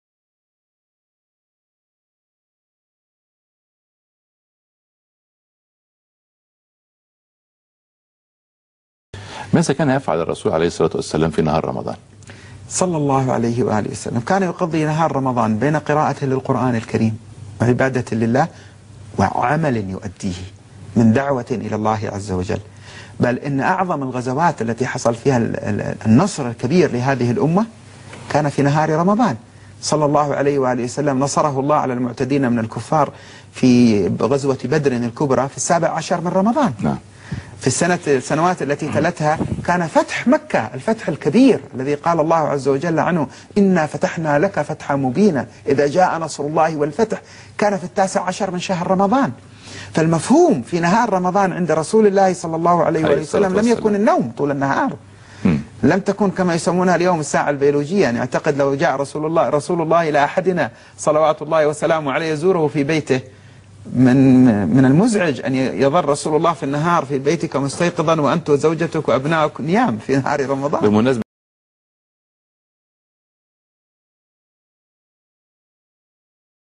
ماذا كان يفعل رسول الله صلى الله عليه وسلم في نهار رمضان
في دقيقة واحدة يبين الحبيب علي الجفري كيف كان يقضي رسول الله صلى الله عليه وسلم نهار رمضان